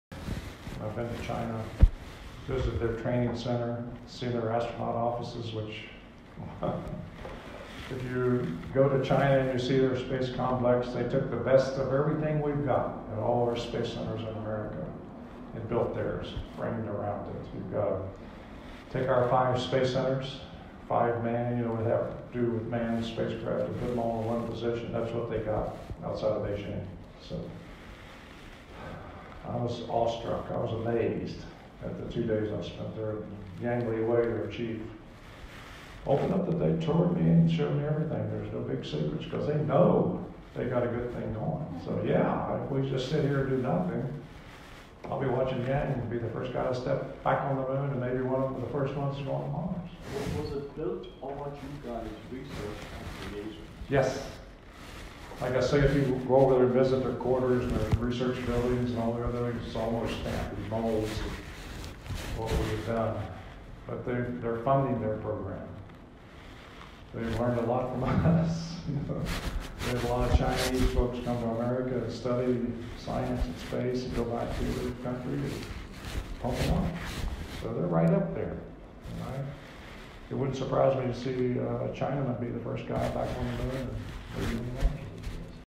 Jon McBride astronaut Admiration for Chinese space programme
Astronaut Jon McBride and Philip Britton of Kennedy Space Centre hosted 36 trade and media at the Blue Room in the Dean, February 7 2019